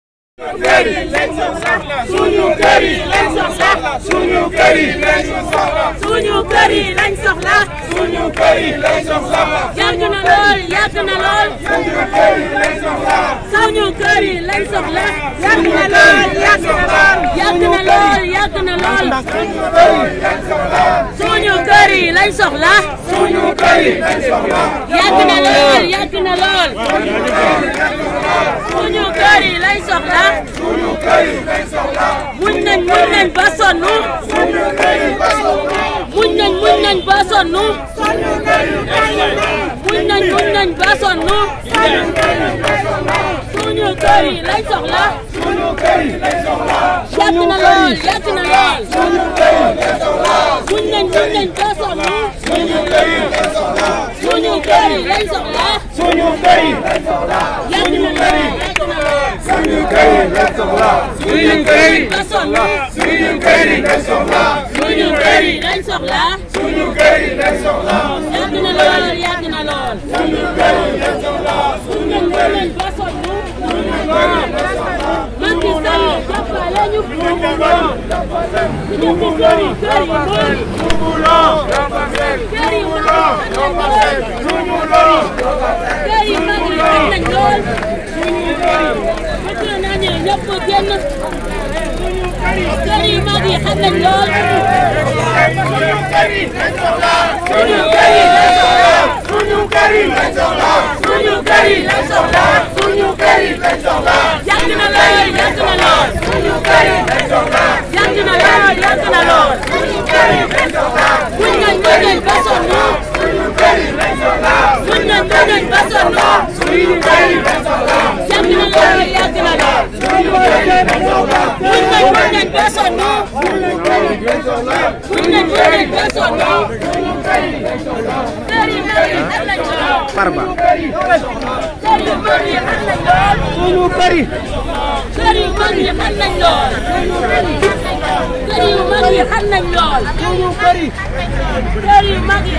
13 juin 2021
Litige foncier dans la Commune de Ndiaffate : les populations marchent contre le maire.